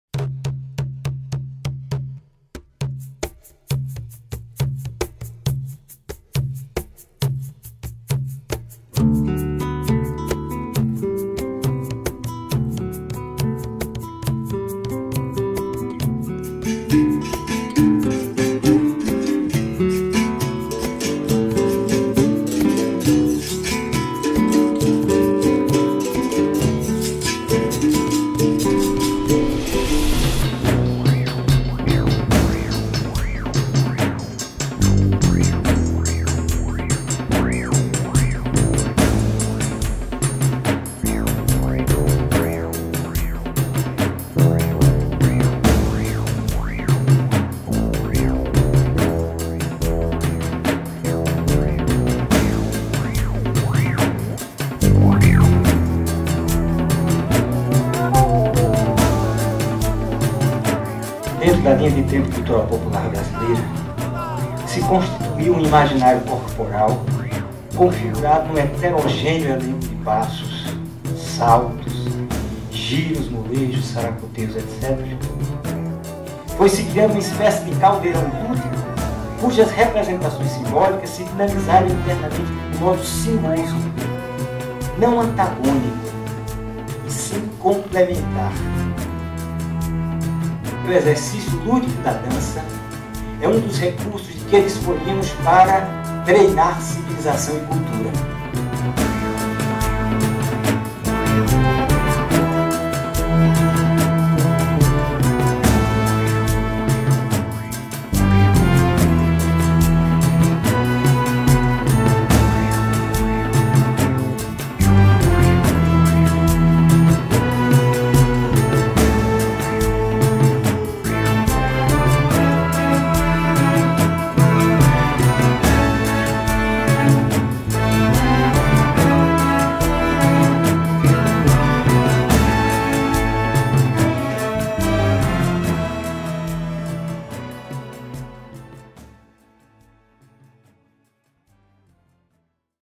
Com Passo Sincopado HD
Antonio Nóbrega apresenta sua visão sobre uma dança brasileira contemporânea sistematizada a partir do encontro de matrizes corporais populares com princípios técnicos, práticas e procedimentos formais provenientes das várias linguagens de dança tanto do Ocidente quanto do Oriente. Ainda tomando a dança no Brasil como ponto de partida de estudo e reflexão, Nóbrega esboça uma interpretação da cultura brasileira, levando em conta a sua ascendência ocidental ou europeia – classe dominante, letrada – e  a popular,  marginal e de substrato oral.